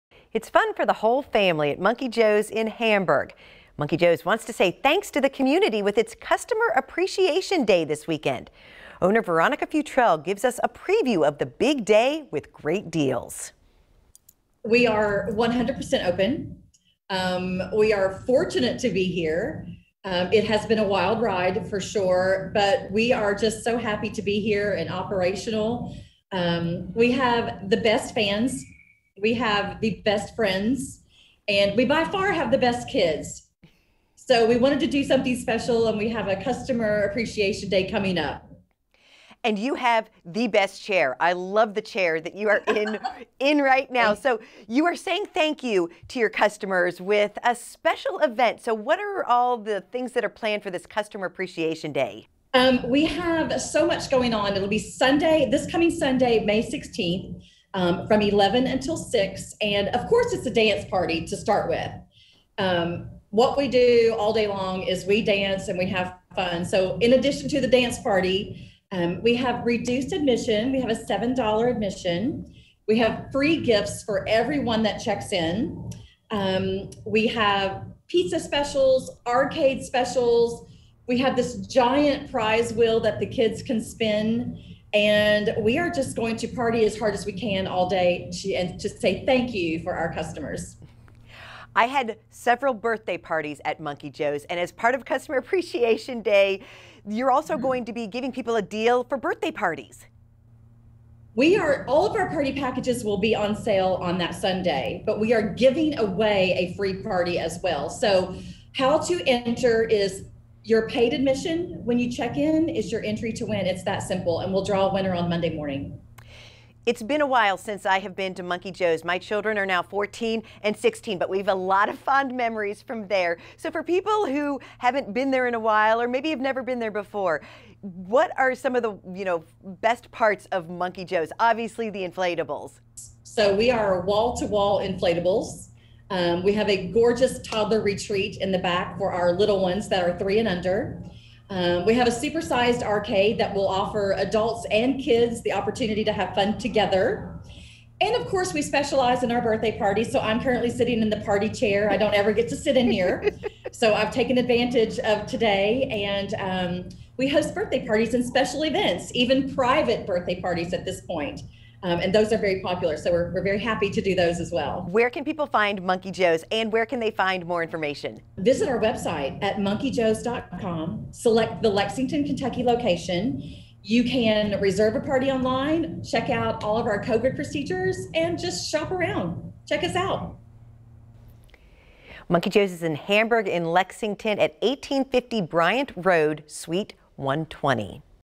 Customer Appreciation Day at Monkey Joe's
Monkey Joe's in Lexington is saying thanks to the community with its Customer Appreciation Day. Admission is $7 per person on Sunday, May 16 from 11:00 a.m. until 7:00 p.m. Owner Veronica Futrell gives us a preview of the big day with great deals and giveaways. for more information.